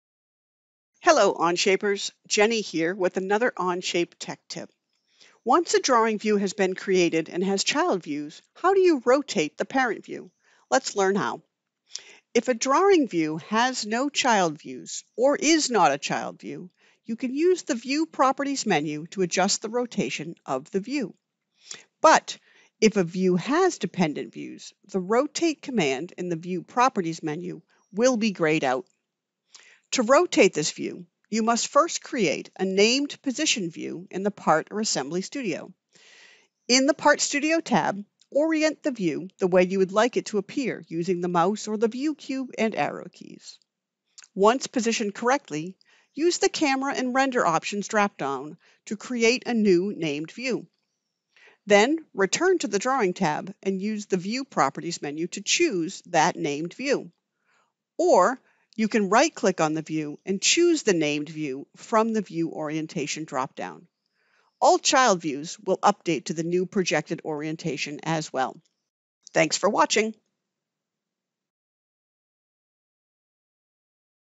Tech Tip: How to Rotate a Parent Drawing View in Onshape
In this Tech Tip, learn how to rotate a view whether an Onshape Drawing has a child view or not – and speed up the drawing process.